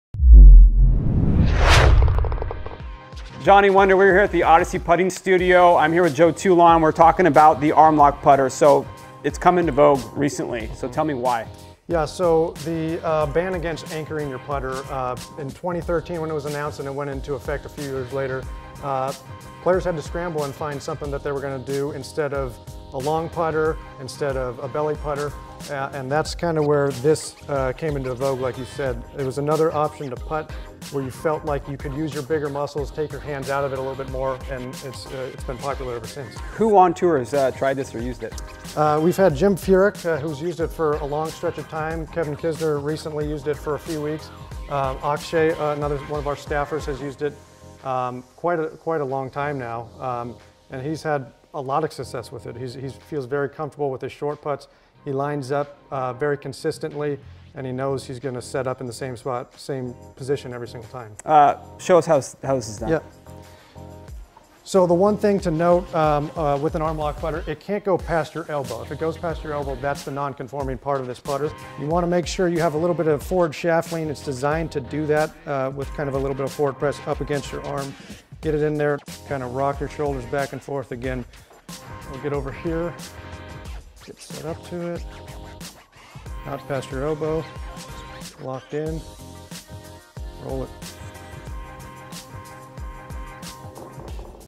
Odyssey 2-Ball Ten Arm Lock Putter | How to Arm Lock
Joe Toulon from Odyssey & Johnny Wunder (GolfWRX) discuss one of the hottest putting trends on Tour, Arm Lock. Joe also demonstrates how to putt with an Arm Lock putter.

Since the anchoring ban a few years ago, golfers have been looking to find alternative methods to improve their putting. Arm Lock is the hottest alternative putting option on Tour, and it's an innovative, conforming design that still retains the benefits of an anchored putter.  Odyssey are bringing all of these performance benefits to their new 2-Ball Ten head shape that’s incredibly popular on Tour.

The 2-Ball Ten Arm Lock features an elongated shaft and 7 degrees of loft that allow the shaft to rest against the inside of the golfer's left arm without anchoring to your body. And the mallet head provides legendary 2-Ball alignment and a super high MOI shape to help improve your performance.

FEATURES & BENEFITS
✅ Legendary Alignment and Shaping
Odyssey have enhanced The Ten shape making it sleeker, more forgiving and engineered to help you frame the ball at address with your intended line.
✅ Adjustable Weighting
The sole of the Arm Lock putters are designed with adjustable weight screws to enhance stability for a more consistent stroke. Two 15 gram weight screws are included with the putter.
✅ Arm Lock Shaft
The Arm Lock putter is engineered with a lengthened shaft to retain the performance benefits of an anchored stroke. Available in 40” and 42”.